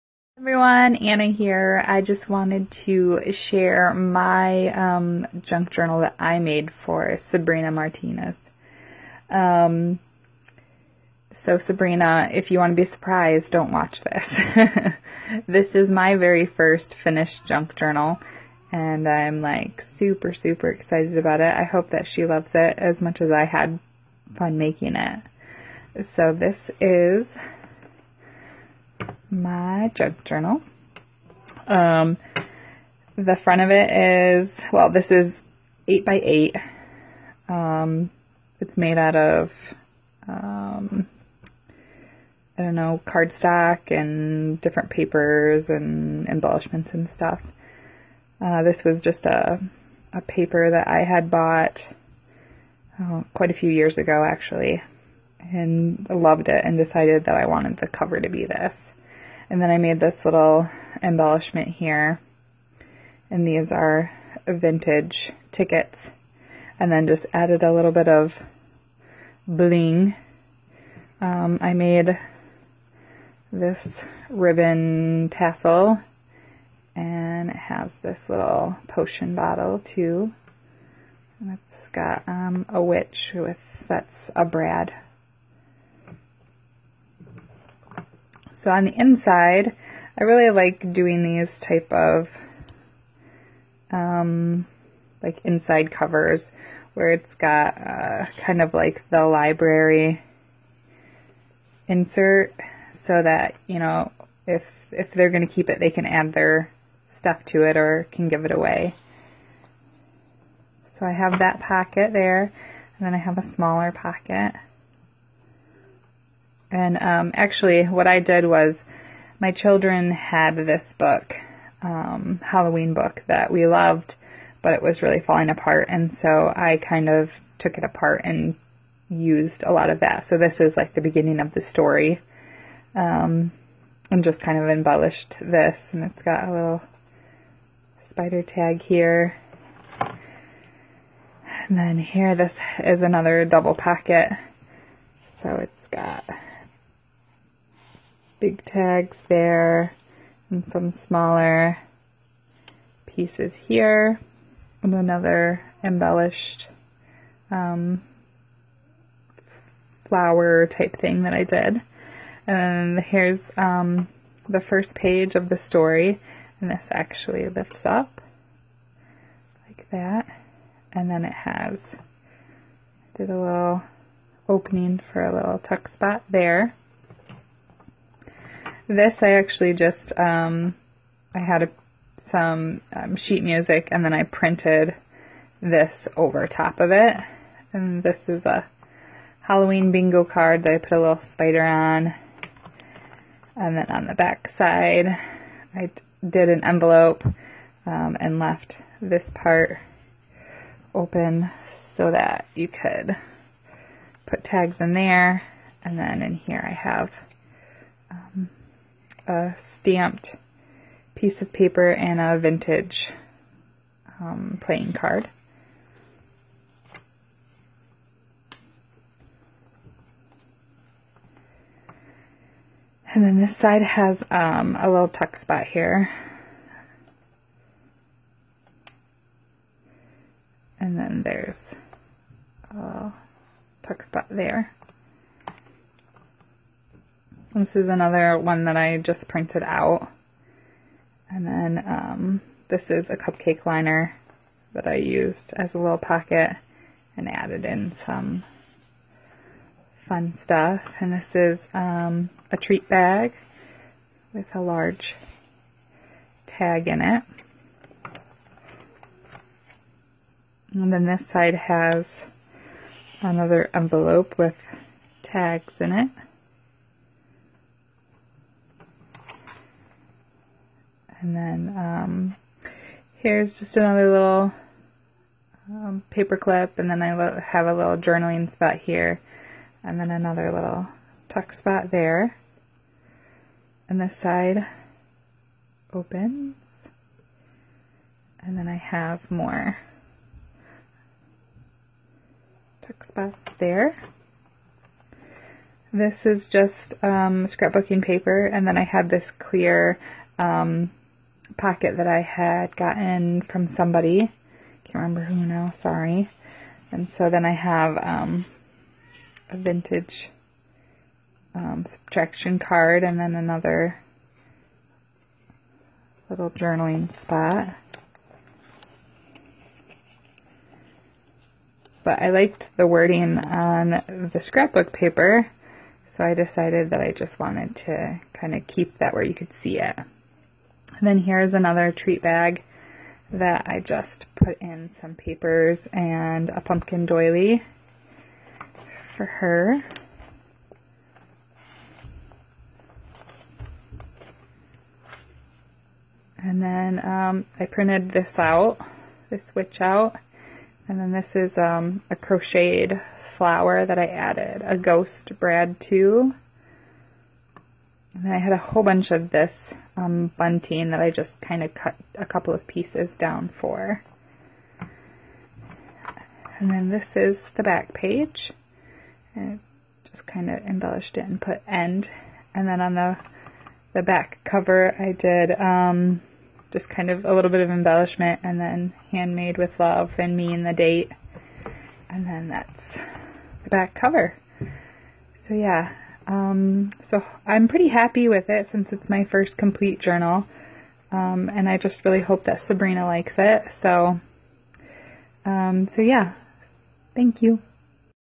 Halloween Junk Journal (My first finished JJ)
Halloween Junk Journal I made last year for a private swap.